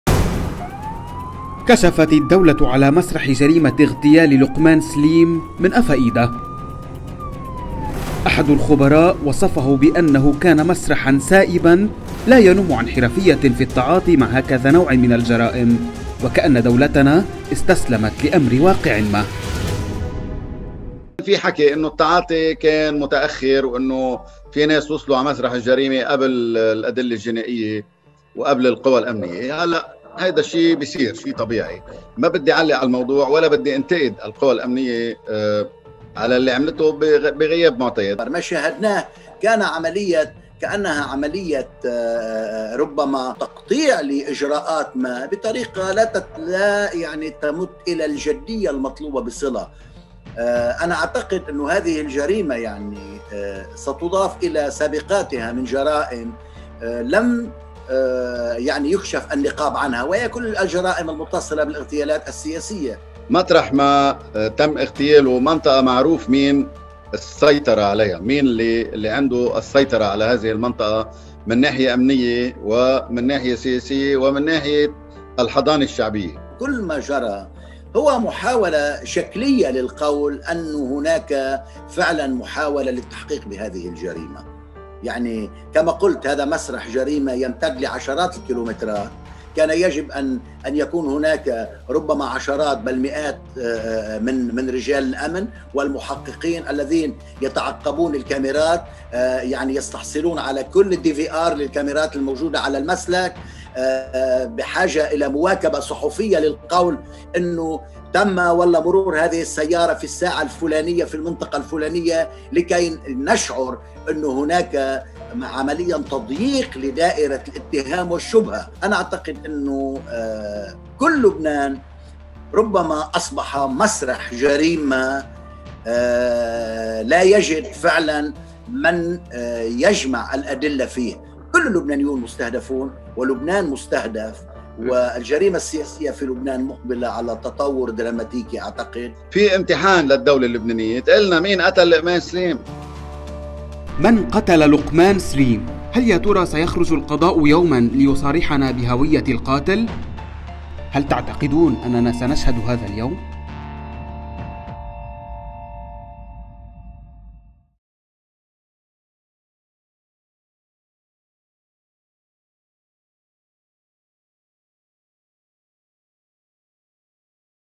مسرح جريمة إغتيال لقمان سليم "سايب"
سويعاتٌ قليلةٌ فقط كلفت الدولة للكشف على مسرح جريمة لقمان سليم، أي بالعاميّة: "من قفا إيدا". وقد وَصف أحد الخبراء هذا المكان بأنه كان "سائبًا"، لا ينمُّ عن حرفيّةٍ في التعاطي مع هكذا نوعٍ من الجرائم، وكأن دولتنا استسلمت لأمرِ واقعٍ ما وعلمت بأن القاتل لن يُكشف، فلم تتعب نفسها كثيرًا.